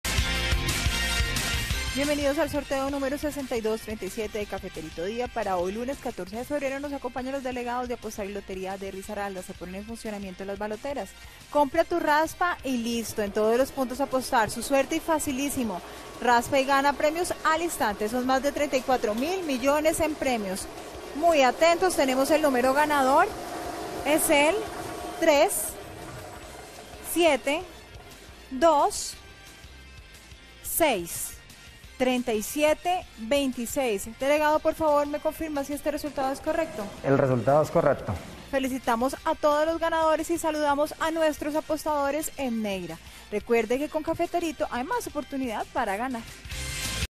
EL CAFETERITO TARDE Resultado del LUNES 14 de febrero de 2022 NÚMERO GANADOR ✅🥇🔥💰
Hola buenas tardes, te presentamos los resultados de las loterías y chances en Colombia, vamos con el Resultados del Chance Cafeterito Tarde del LUNES 14 de febrero de 2022.